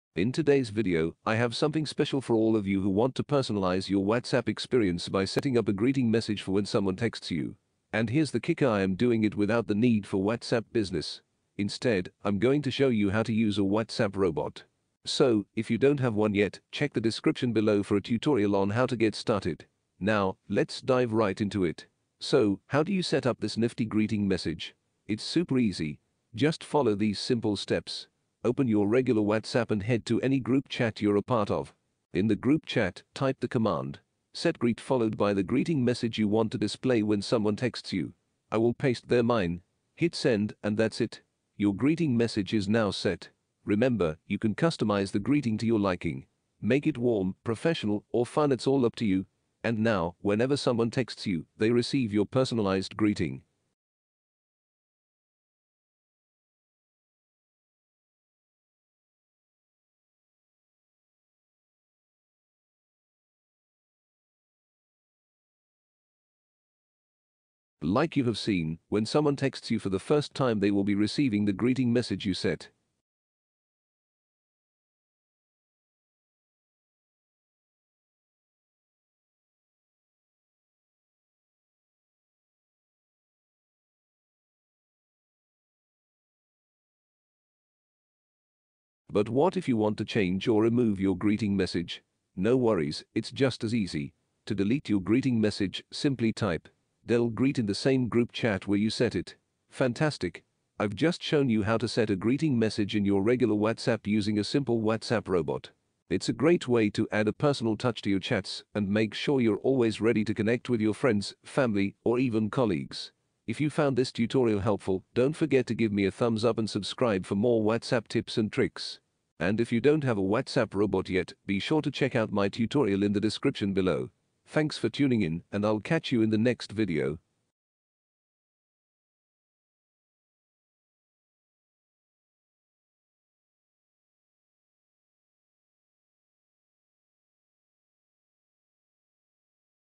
Easy WhatsApp Bot Greeting Message Setup!
Stop manually greeting every new contact! Learn how to easily set up automated greeting messages using a WhatsApp Bot. This step-by-step guide will show you how to create a welcoming first impression and save valuable time.